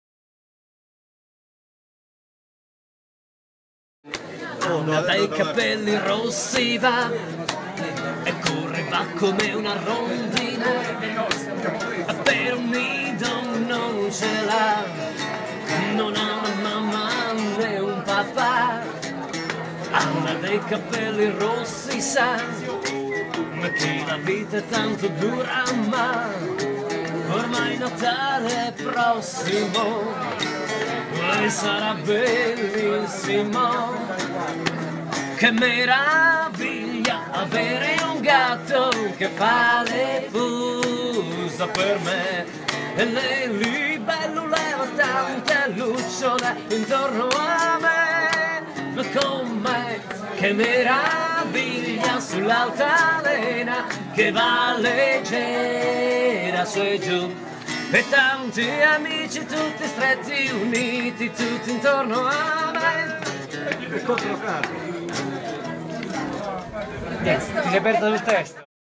Serpieri, i Raggi Fotonici e...una fantastica interpretazione di un classico
clip musicale "rubata" a Diamante, il fumettista Paolo Eleuteri Serpieri alla chitarra con Mirko Fabbreschi e Laura Salamone (Raggi Fotonici) in una pausa al Peperoncino Festival del 2009 (con tutta la carovana dal mitico Aligia)